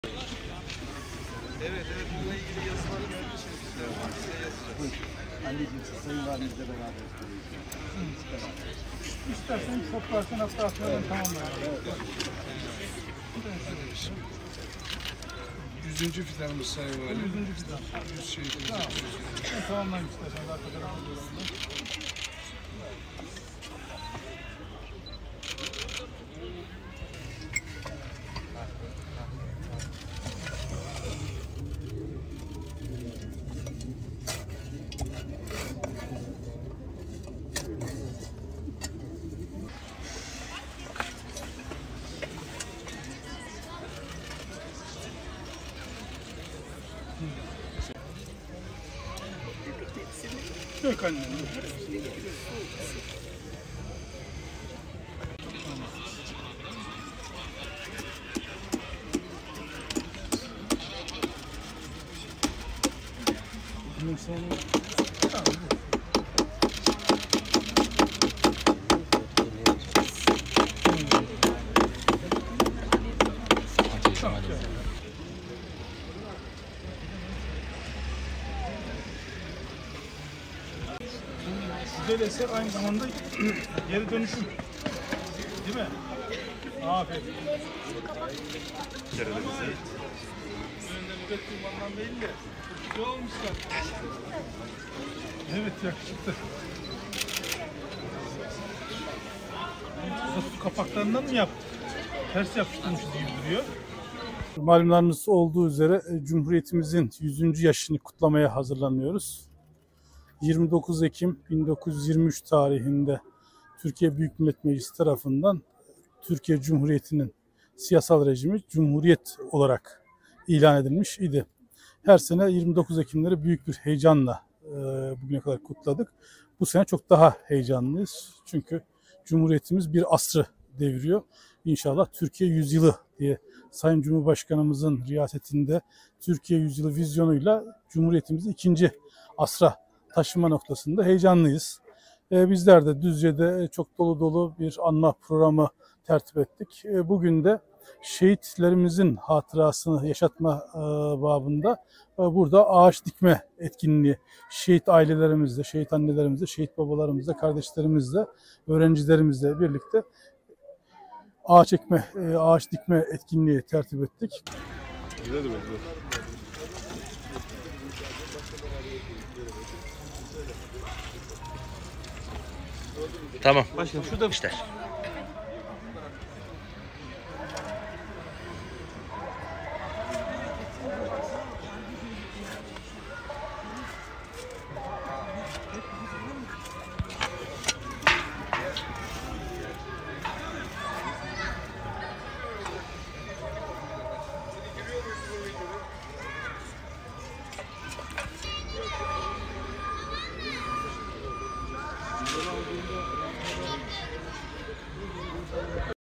Düzceli 100 şehit için 100 fidan dikildi
Cumhuriyet'in 100. yılında Düzceli 100 şehit adına 100 fidan toprakla buluşturuldu.
Şehit Teğmen Uğur Altan İlkokulu’nda Düzceli şehitler için 100 fidan dikim etkinliği gerçekleştirildi. Düzce Valisi Selçuk Aslan’ın da katılım sağladığı programda öğrencilerin geri dönüştürülmüş ürünlerden hazırladığı bayrak sergisi dikkat çekti. Kapaklar, kaşıklar ve köpüklerden oluşturulan bayrak sergisinin ardından Vali Selçuk Aslan ve beraberindeki heyet fidan dikimi gerçekleştirdi. Şehit aileleri de vatan için feda ettikleri çocukları için birer fidan dikerek Cumhuriyet'in 100. yılı etkinliklerine destek verdi.

"Bu sene daha heyecanlıyız"
Cumhuriyet’in 100. yılında daha da heyecanlı olduklarını belirten Düzce Valisi Selçuk Aslan, "Cumhuriyet'in 100. yaşını kutlamaya hazırlanıyoruz. 29 Ekim 1923 tarihinde Türkiye Büyük Millet Meclisi tarafından Türkiye’nin siyasal rejimi Cumhuriyet olarak ilan edilmiş idi. Her sene 29 Ekimleri büyük bir coşku ile kutladık. Bu sene çok daha heyecanlıyız. Cumhuriyetimiz bir asrı deviriyor. Sayın Cumhurbaşkanımızın riyasetiyle Türkiye yüzyılı vizyonu ile Cumhuriyetimizi ikinci asra taşıma noktasında heyecanlıyız. Bizlerde Düzce’de programlar tertip ettik. Şehitlerimizin isimlerini yaşatmak için Şehit Teğmen Uğur Altan İlkokulu’nda şehit ailelerimiz ile birlikte fidan dikim etkinliği gerçekleştirdik" ifadelerini kullandı.